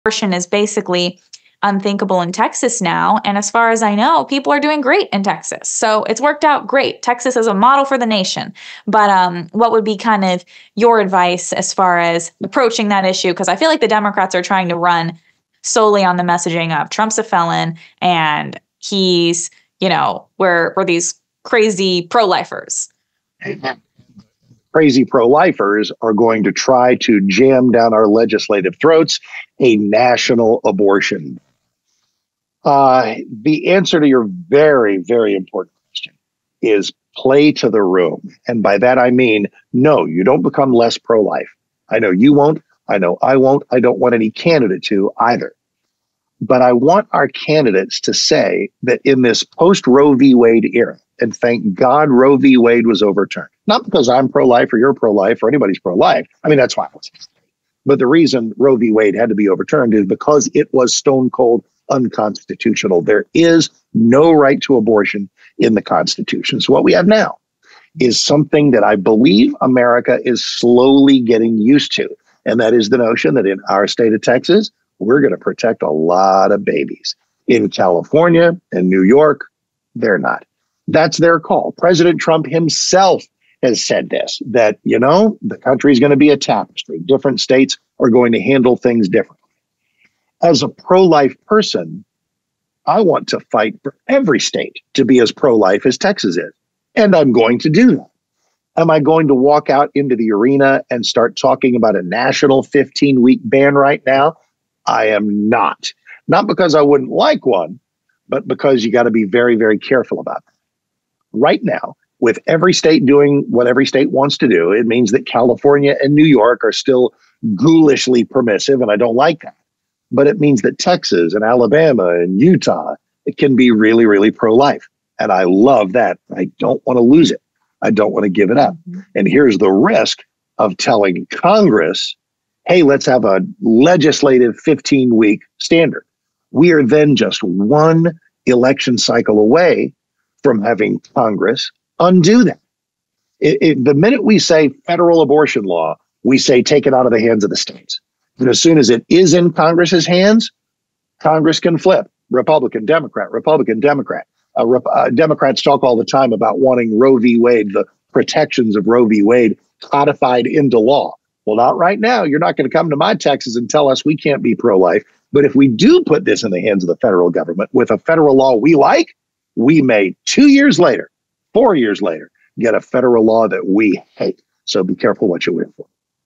Radio Host Mark Davis Discusses How Republicans Should Handle The Abortion Debate
Radio host Mark Davis offers his perspective on how Republicans should handle the topic of abortion across the country